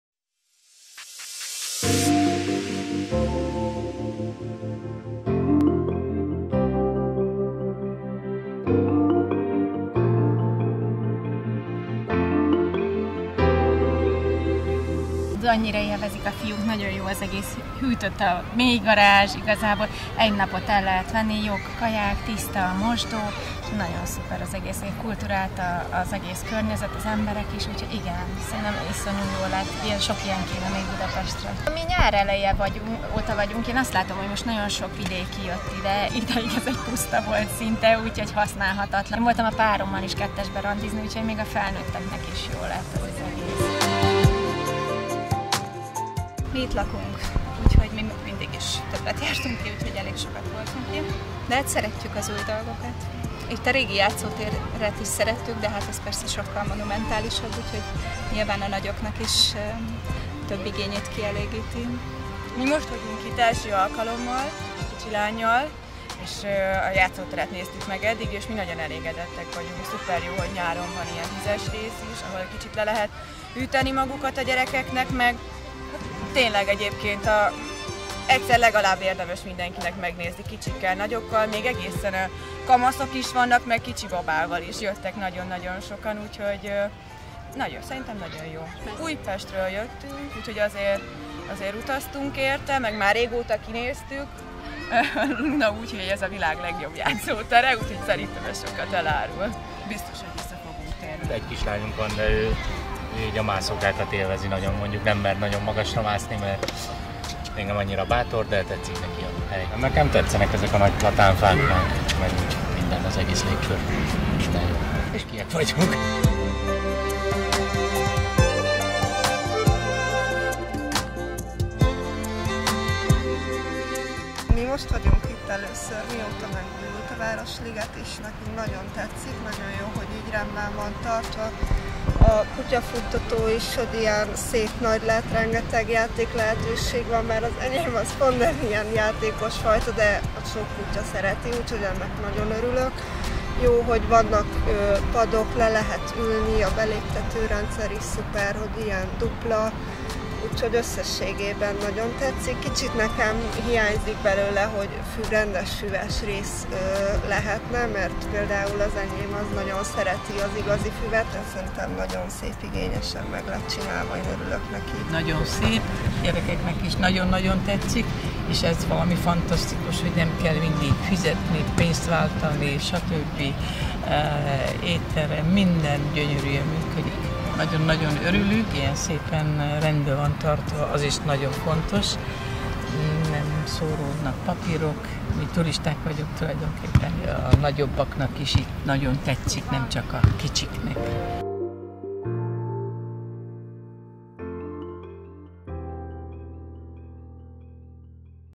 Vidékről is feljönnek a megújult Ligetért
A felújított Városliget minden részén találni valami újat, izgalmasat. Tiszta környezet, modern játékok és rendezett park. Ezeket kínálja nekünk most, és az emberek nagyon elégedettek a végeredménnyel. Találkoztunk olyan látogatókkal is, akik vidékről jöttek megnézni, mivé lett most a Városliget.